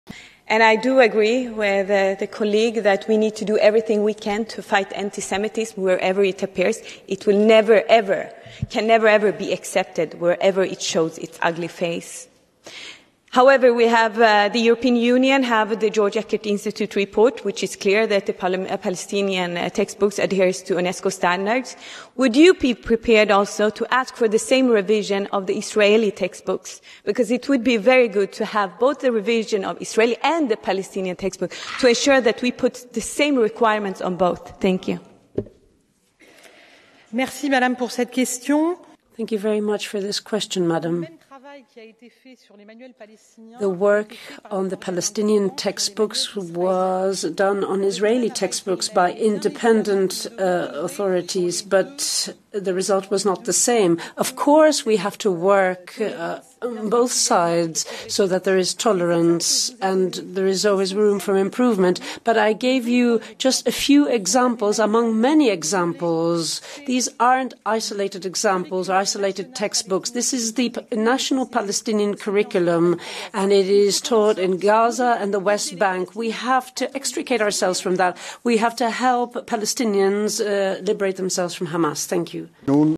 Palestinian textbooks need the same revision as the Israeli textbooks! Evin Incir EU debates!
Parliament has condemned Hamas’ attacks against Israel while expressing serious concern over the humanitarian situation in the Gaza Strip. In the resolution adopted in October with 500 votes in favour, 21 against and 24 abstentions, MEPs strongly condemn the brutal attacks, express their support to Israel and its people and underline the need to “eliminate the terrorist organisation Hamas” .

They also call for the immediate release of all hostages kidnapped by Hamas and recognise Israel’s right to self-defence “as enshrined in and constrained by international law” . As such, any actions by Israel must strictly comply with international humanitarian law, the text states.

Parliament also stresses that both the attacks by Hamas and the Israeli response risk reinforcing a cycle of violence in the region. MEPs therefore call for a “humanitarian pause” of the fighting and stress that attacking civilians and civilian infrastructure, including UN workers, medical workers and journalists, is a serious violation of international law.

Following Hamas’ deadly attack against Israel on 7 October 2023, which claimed the lives of around 1 200 people and resulted in more than 200 people being taken hostage, Israel declared war against the terrorist group. It began striking the Gaza Strip from the air, later sending ground forces into the sealed-off coastal enclave. According to the Hamas-run health ministry, more than 14 000 Palestinians have so far been killed.

EU leaders increasingly back a humanitarian cease-fire in Gaza

The dire humanitarian situation in Gaza is prompting more European Union countries to support the idea of a humanitarian cease-fire in the war between Israel and Hamas.